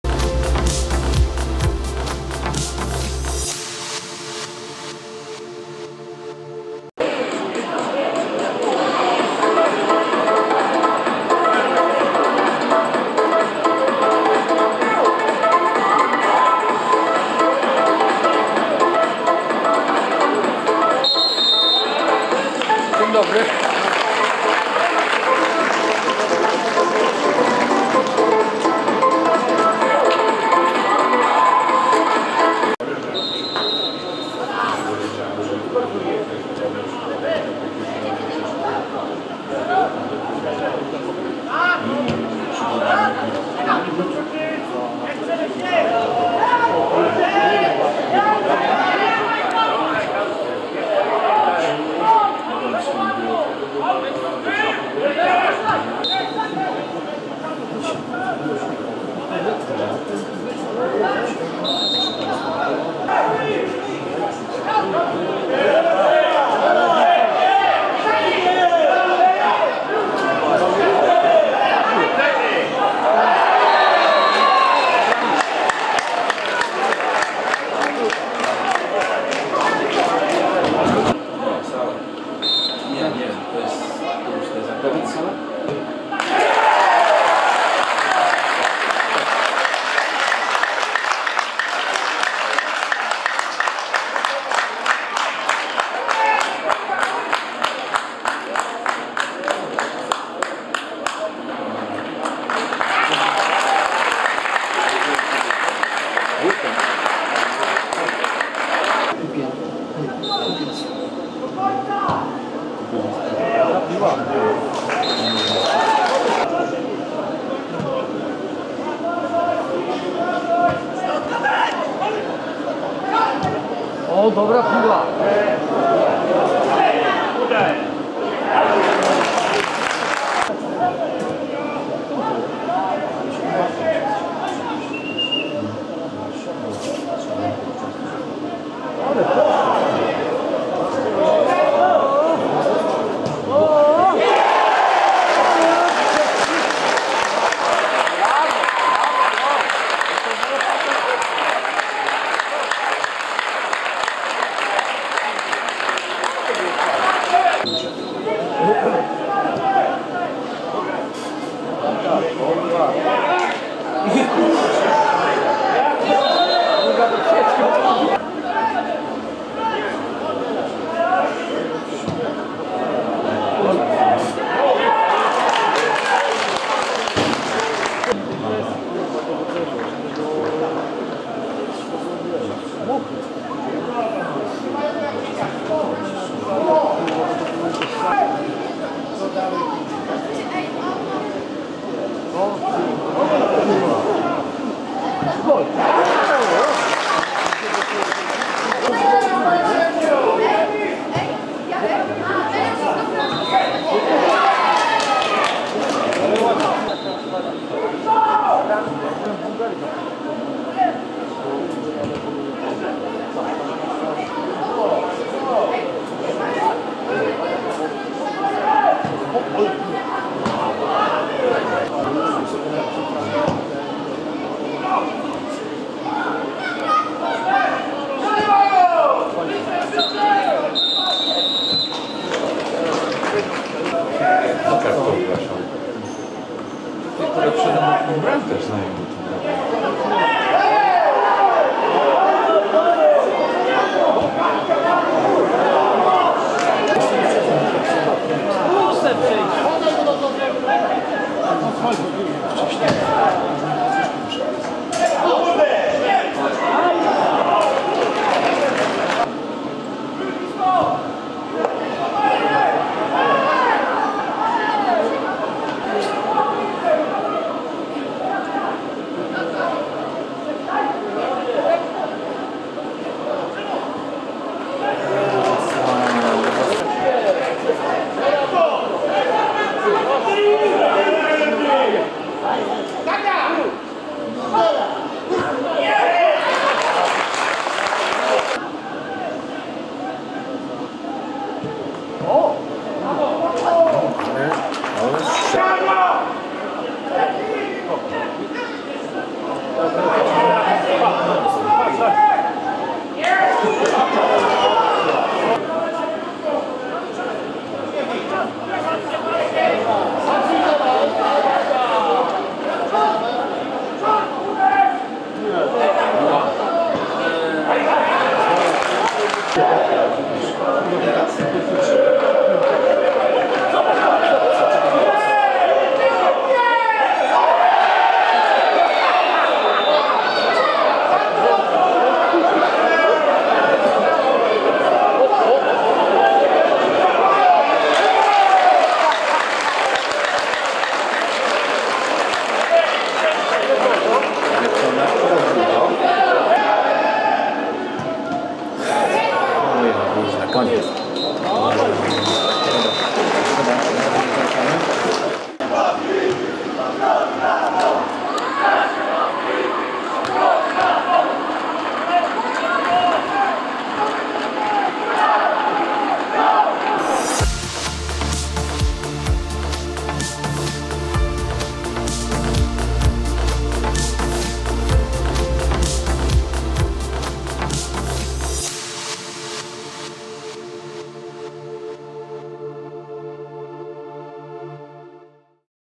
BROŃ RADOM ENERGIA KOZIENICE 2 : 0 Short
BROŃ RADOM - ENERGIA KOZIENICE 2:0 (2:0)
BramkI: Dawid Sala 18 z karnego, Nowosielski 22.

Energia: Kula - Szary, Lament, Janiszewski, Janik, Kalinka, Michalski, Pryciak, Putin, Ciupiński, Kołodziejczyk     Trener  : Grzegorz  Seremak

Broń: Młodziński - Kencel, Kupiec, Sala, Leśniewski, Bojek, Wicik, Nowosielski, Więcek, Nogaj, Czarnecki.